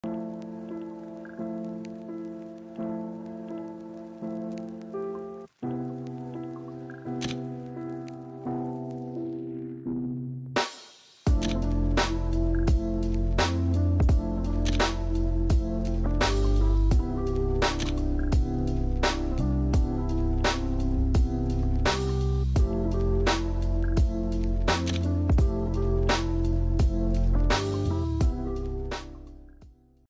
Testing the old FUJIFILM X20 in 2023
Hey everyone, I tested an old Fujifilm camera and was blown away by the results!